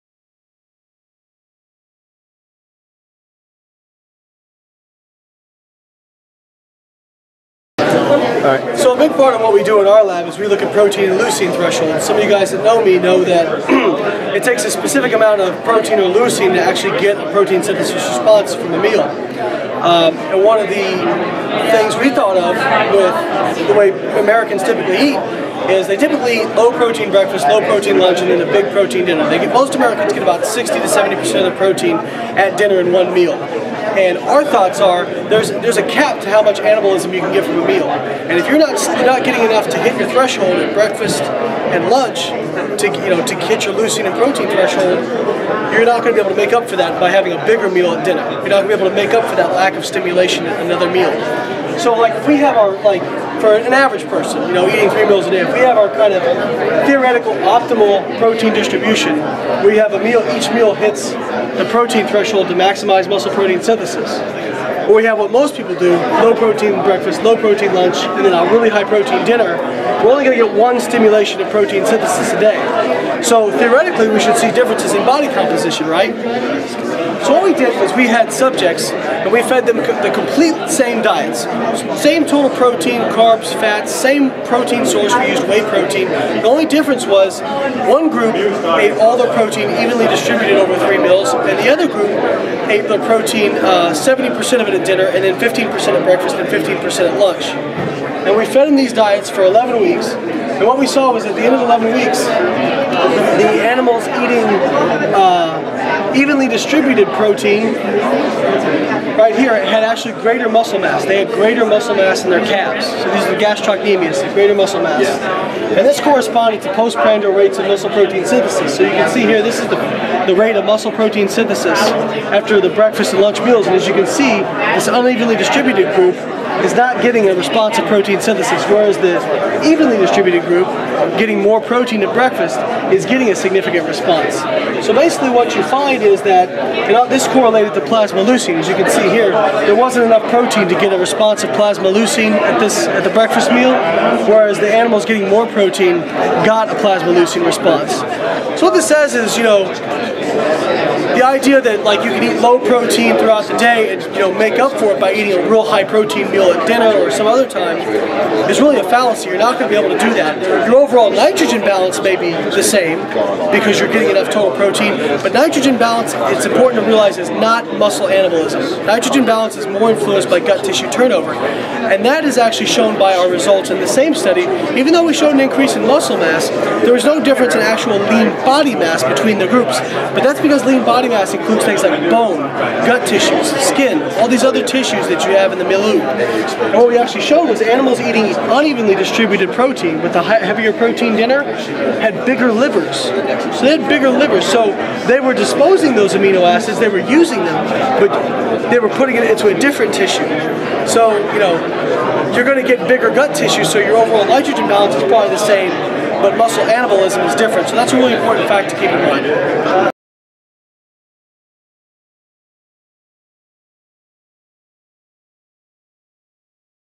Layne Norton 2012 ISSN Research Poster Presentation
Presenting my research post on protein distribution at the 2012 International Society of Sports Nutrition Symposium in Clearwater, FL.  I apologize for how close the video is, it was a very cramped spot and we could not get it any further away.